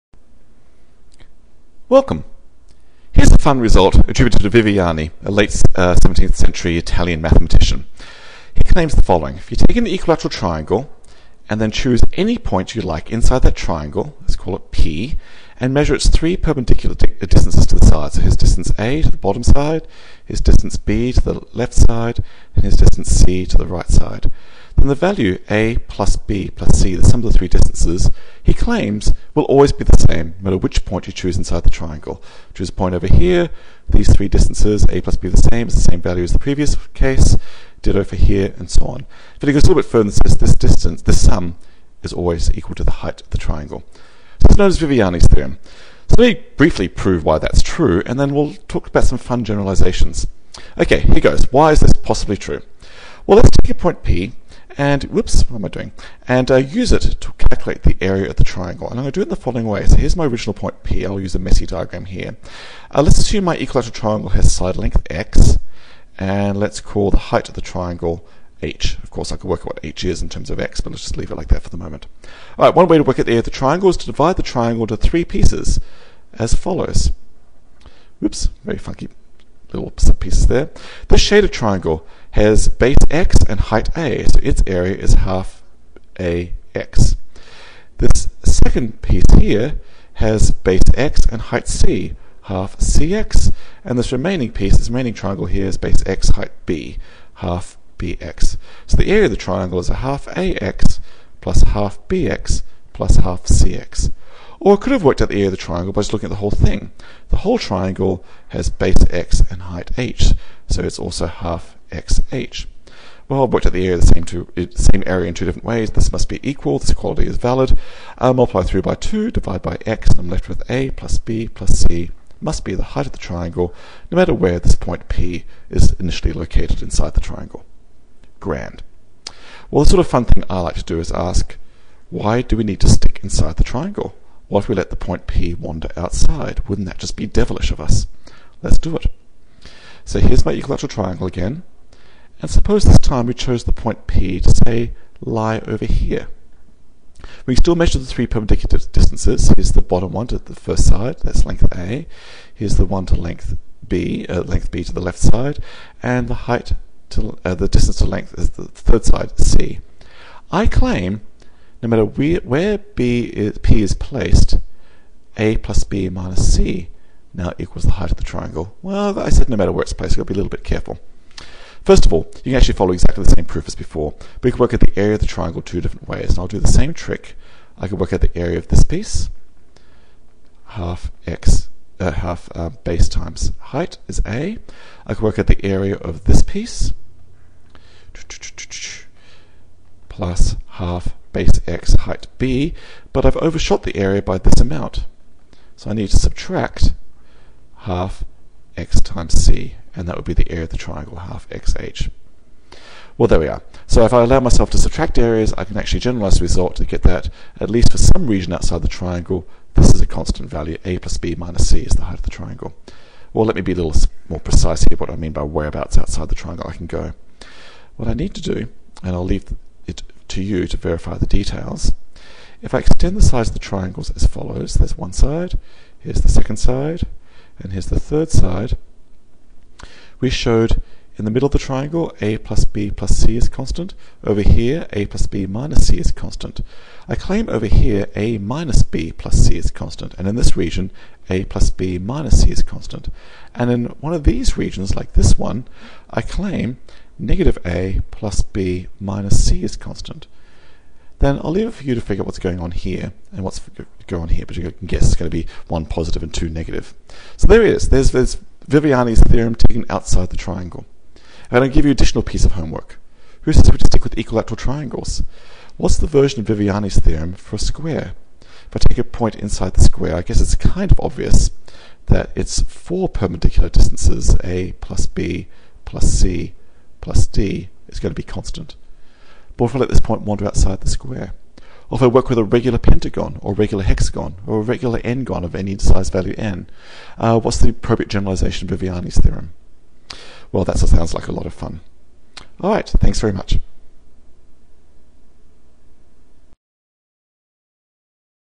Viviani's Theorem (TANTON Mathematics)
Pick a point inside an equilateral triangle and sum its three distances to each of the sides of the triangle. Then this sum has value the height of the triangle, no matter the point initially chosen.

This charmer is attributed to 17th-century mathematician Viviani. Here we prove the result, and then go further and let the point wander outside the triangle, and then go to shapes other than triangles!